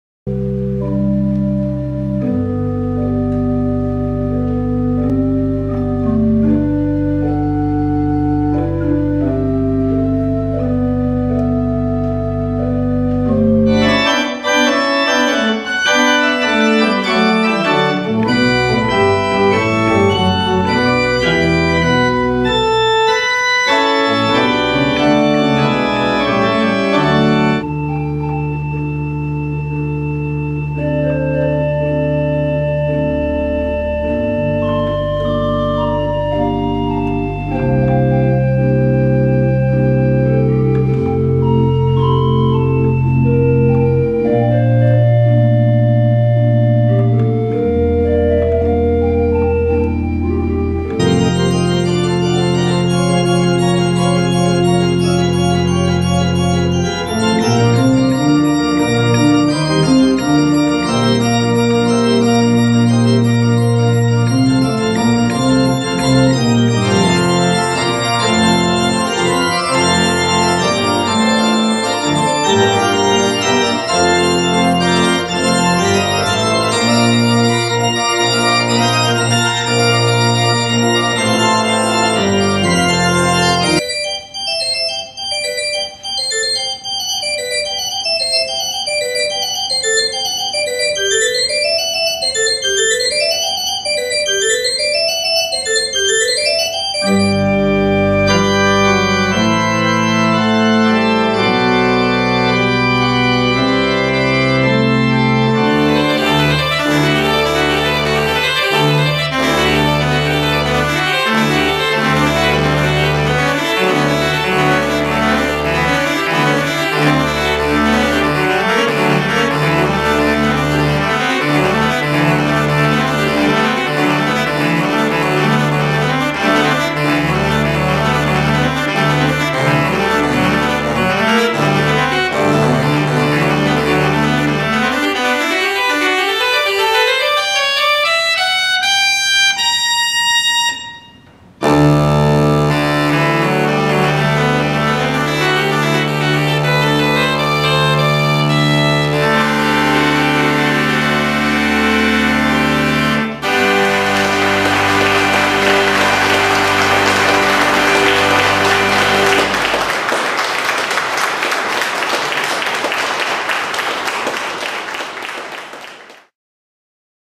Four Organists - One Pipe Organ
The 40th anniversary of the Van Daalen Tracker Organ at Zion Lutheran Church, Hopkins, Minnesota.